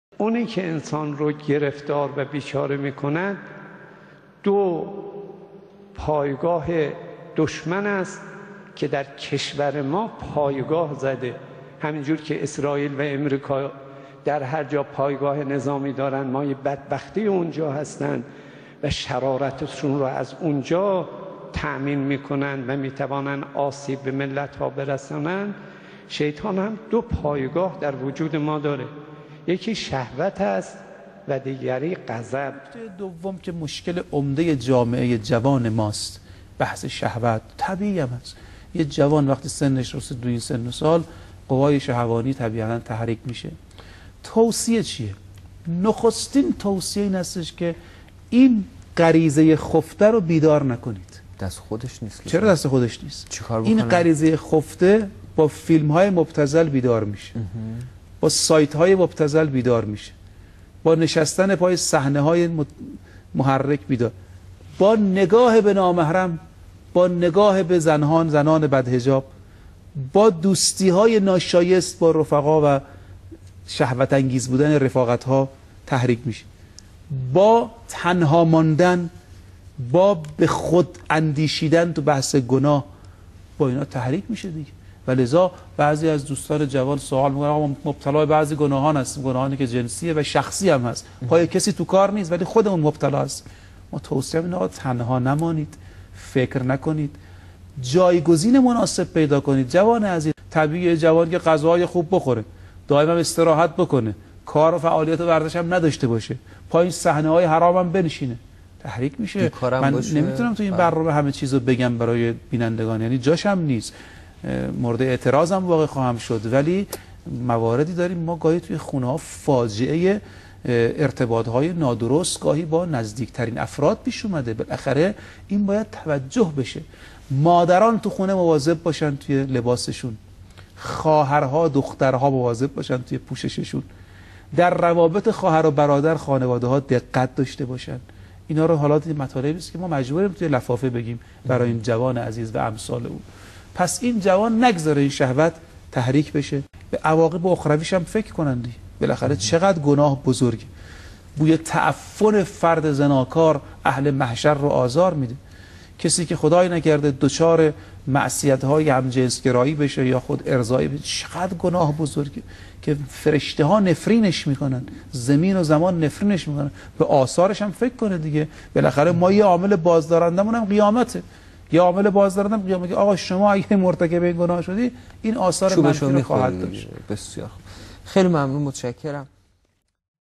A sample of corrupt thinking of religious teachers , how to fight sexual desire
- Ayatollahs are worried of sexual passion of youth and teach them how to fight sexual desire and prevent getting on .